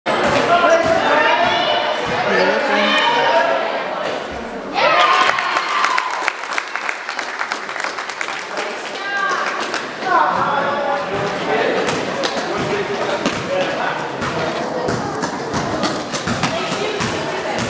Riley's 1st basket Game 7
Riley's first basket in Game 7 of his 2016 season on March 5th.  Unfortunately, they lost 24-22